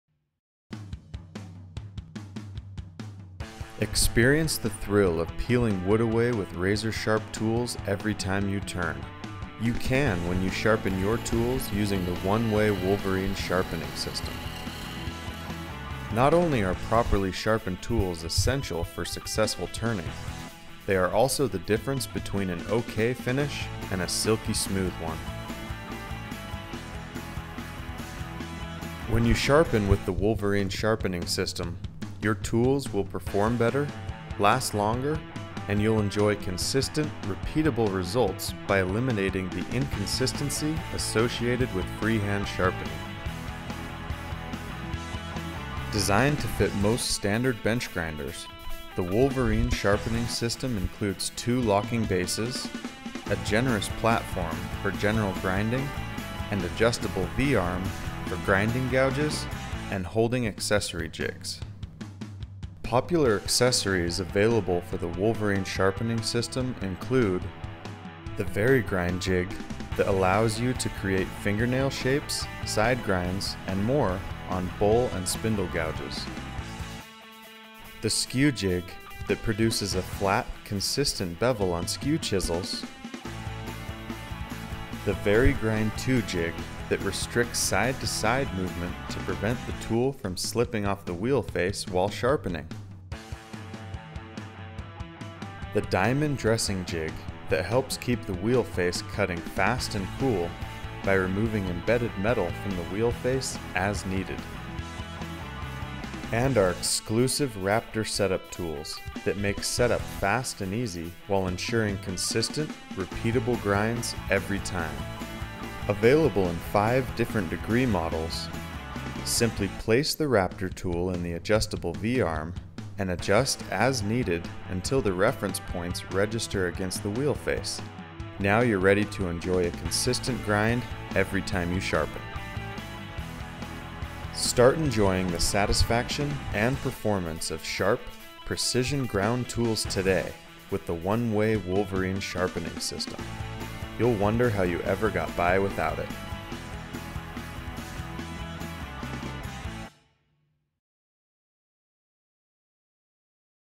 Oneway Wolverine Sharpening System (Woodturning)
Sharpen your woodturning tools accurately with the Wolverine Sharpening System.

The Basic System consists of two bases, V-arm support and the platform. The two bases allow a base to be installed at each wheel on your grinder, so the accessories can be used at either wheel. The V-arm Support supports the handle end of the woodturning or carving tool while the cutting edge is in contact with the grinding wheel. The cutting bevel is determined by moving the V-arm towards or away from the grinding wheel, then holding the V-arm in position by tightening the cam lock. The platform is a 3" x 5" x 1/4" thick steel plate and is used when sharpening skew chisels, wood chisels, scrapers or gouges. The platform is fully adjustable so you can set it to the required angle. Instructions included. Grinder not included.

- SOCIAL MEDIA -